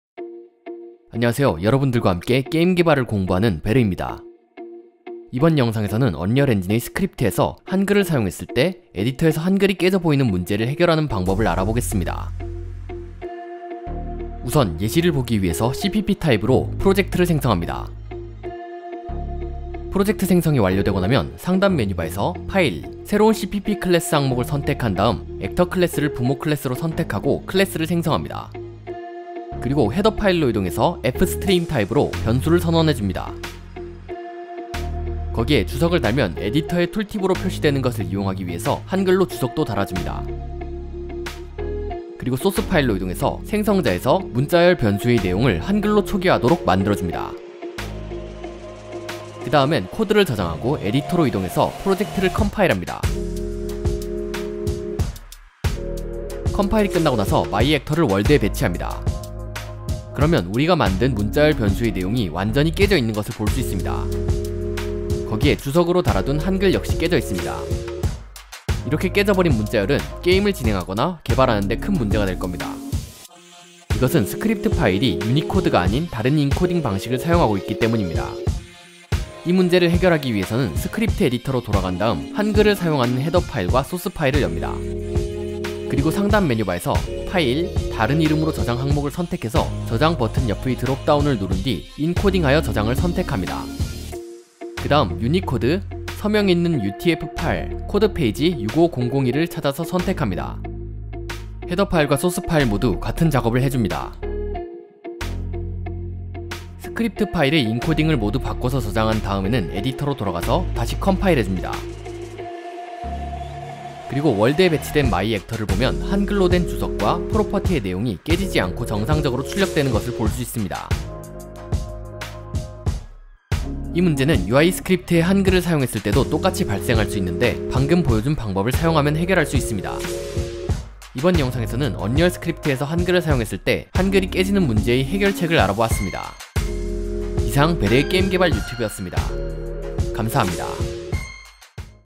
스크립트 한글 깨짐 문제 해결하기 | 언리얼 4
언리얼 스크립트에서 한글을 사용했을 때 한글이 깨지는 문제를 해결하는 방법을 알아봅시다.

개발단에 가입하여 소소한 혜택과 함께 베르의 게임 개발 유튜브를 지원해주세요!

타임라인
0:00 인트로
0:13 한글 깨짐 문제와 해결법
2:03 아웃트로